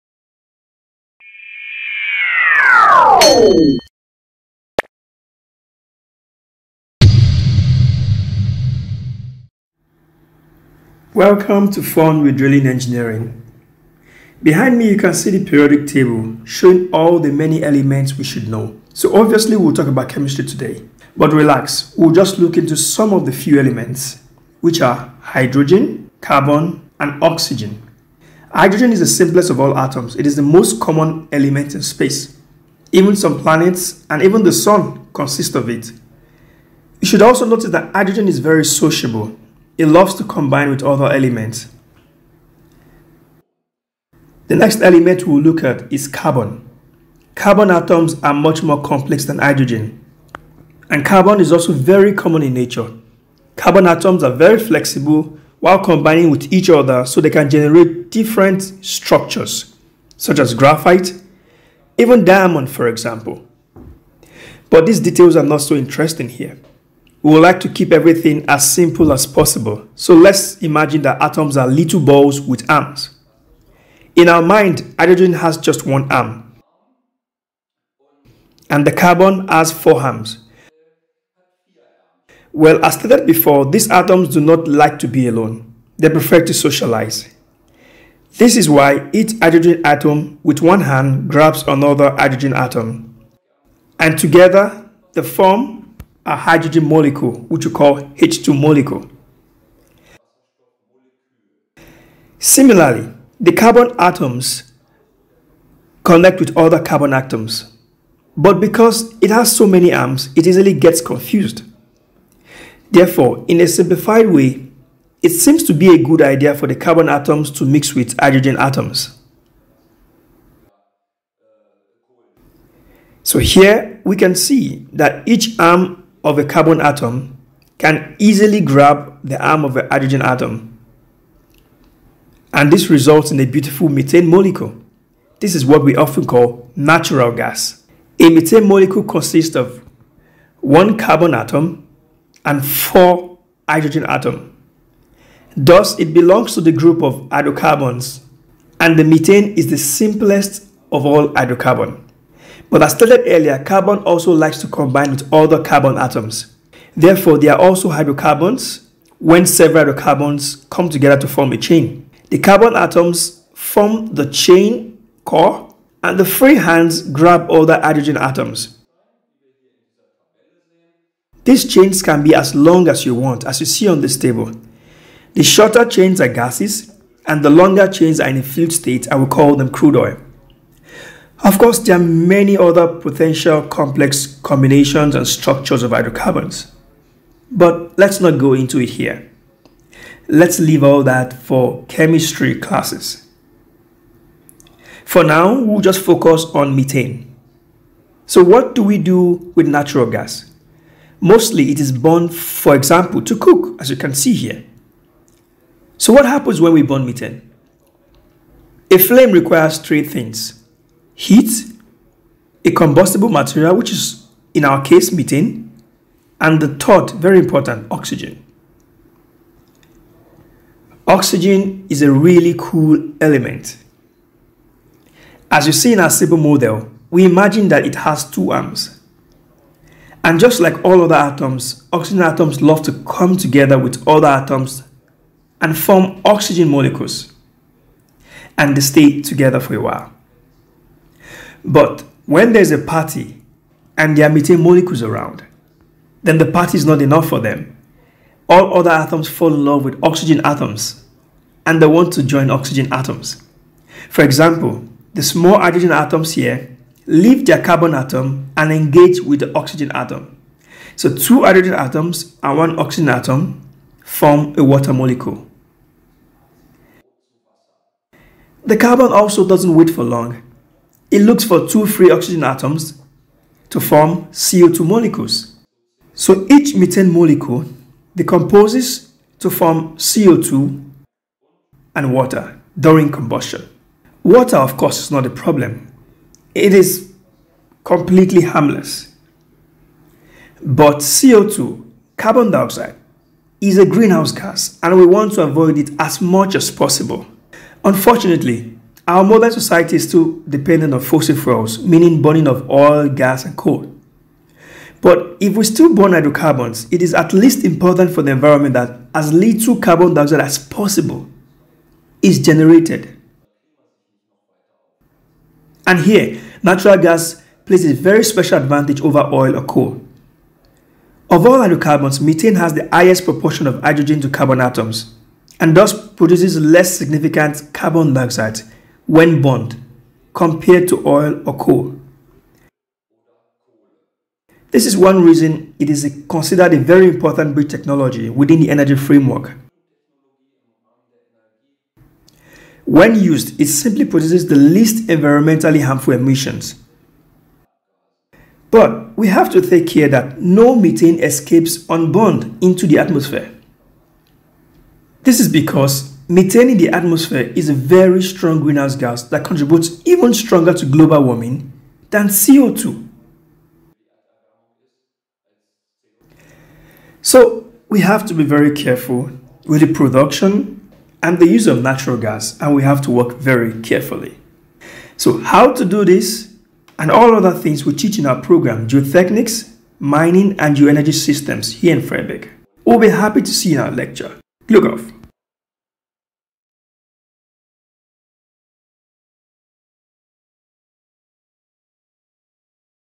Is natural gas really eco friendly? And why?
Camera, cut and atom models: Susann Klein
Made at the Institute of Drilling Engineering and Fluid Mining of the Technical University Bergakademie Freiberg, Germany.
The education of Petroleum Engineers belongs to the program "Geotechnics and Mining".